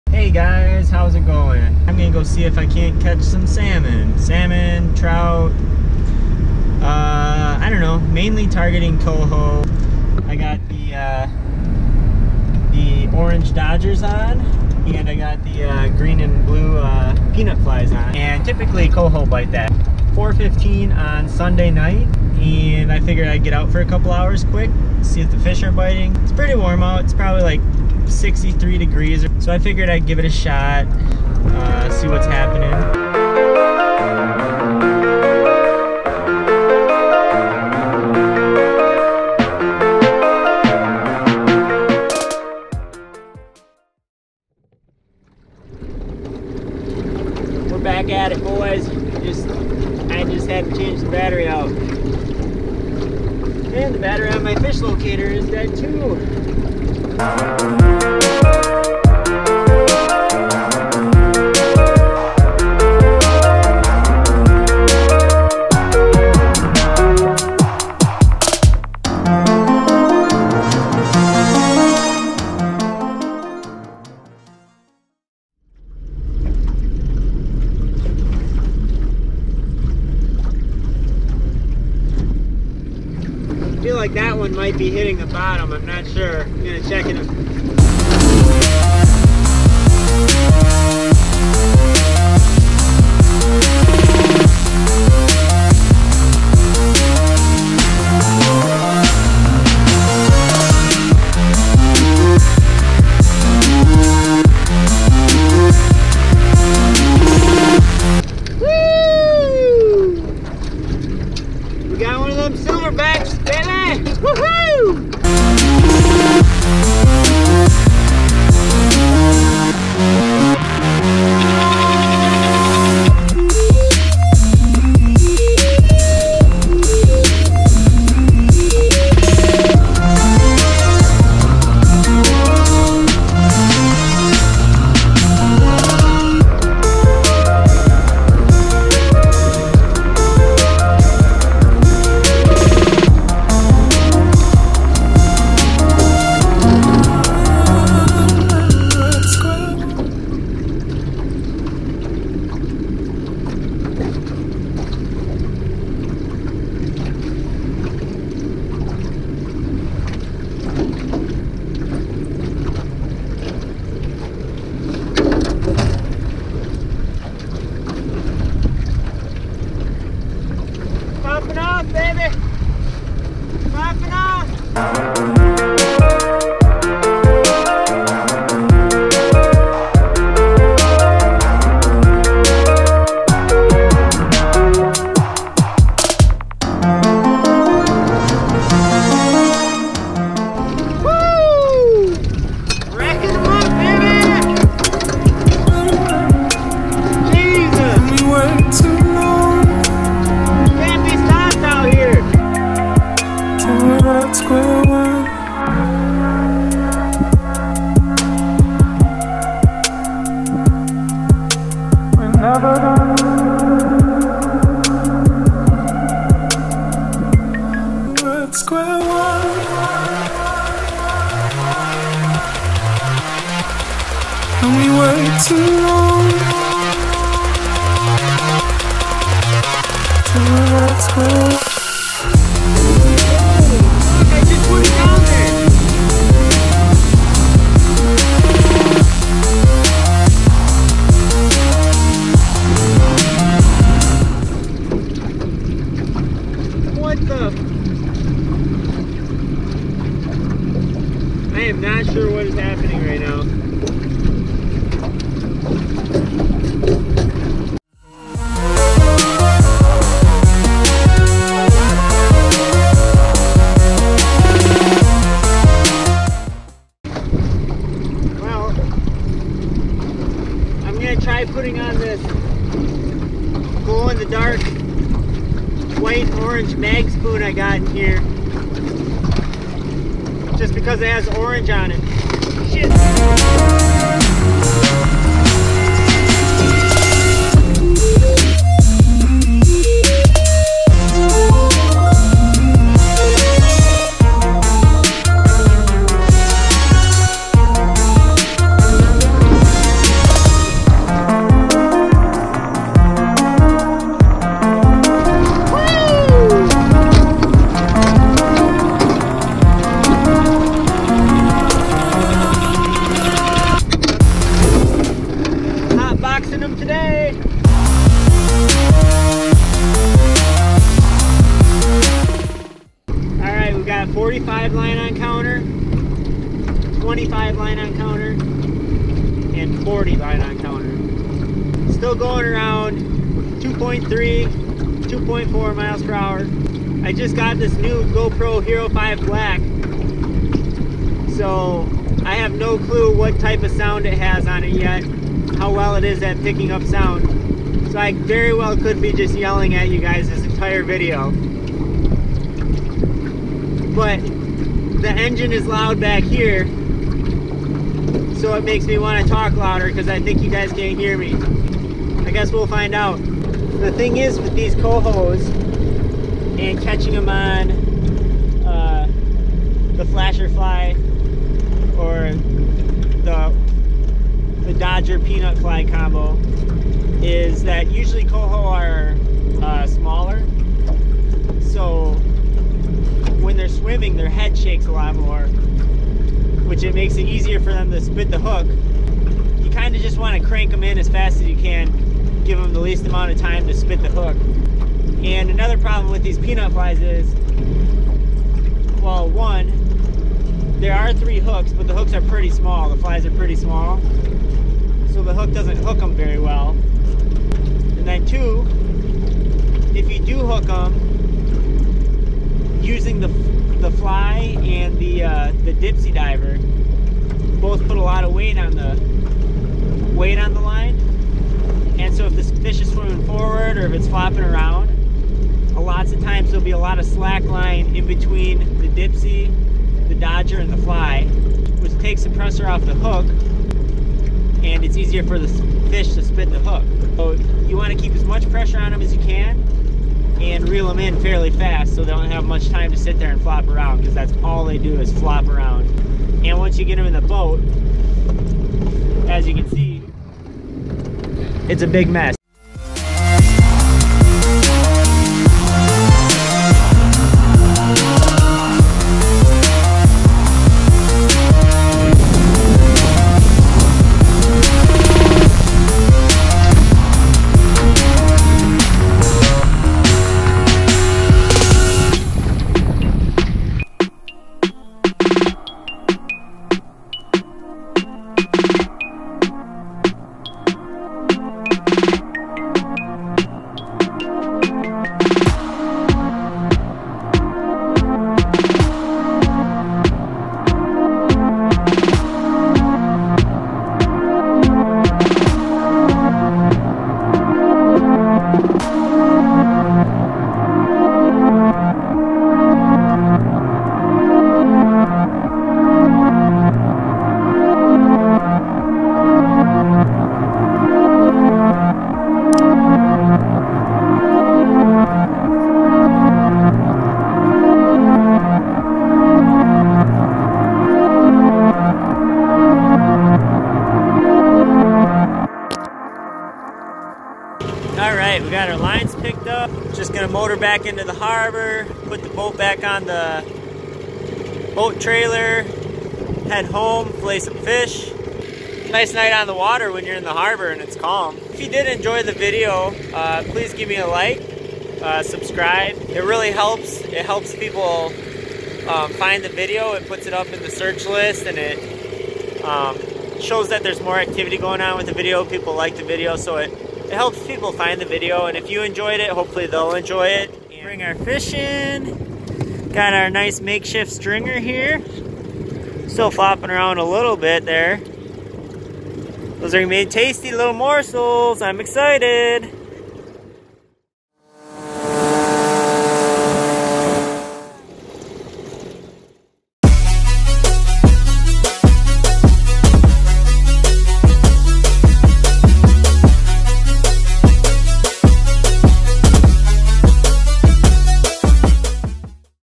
Milwaukee Salmon Fishing Coho 5 13 18
Great day of fishing in the Milwaukee Harbor! Ended up being a little rough out there, but we go the job done!